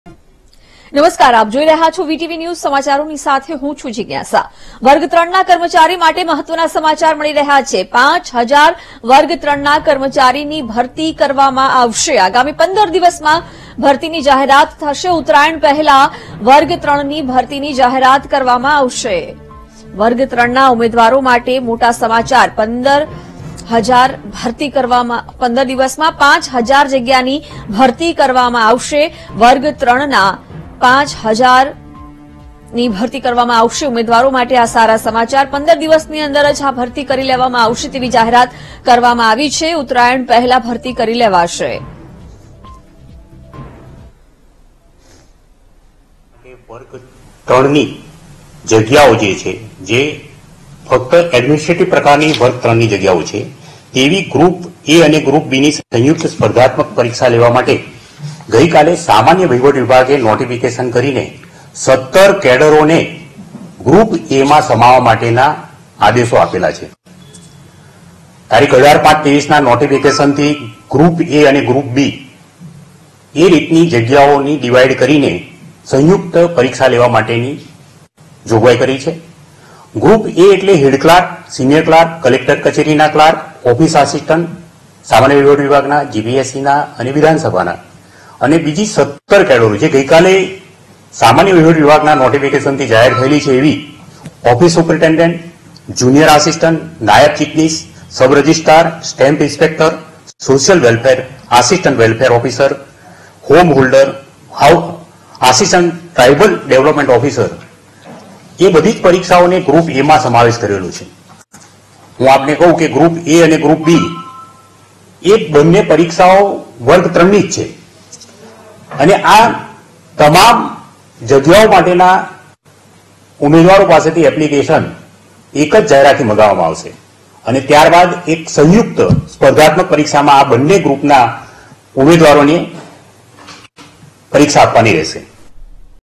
BIG BREAKING | આગામી 15 દિવસમાં વર્ગ-3ની પાંચ હજાર જગ્યાની ભરતીની જાહેરાત થશે
BIG BREAKING | આગામી 15 દિવસમાં વર્ગ-3ની પાંચ હજાર જગ્યાની ભરતીની જાહેરાત થશે: ગૌણ સેવા પસંદગી મંડળના સચિવ હસમુખ પટેલનું એલાન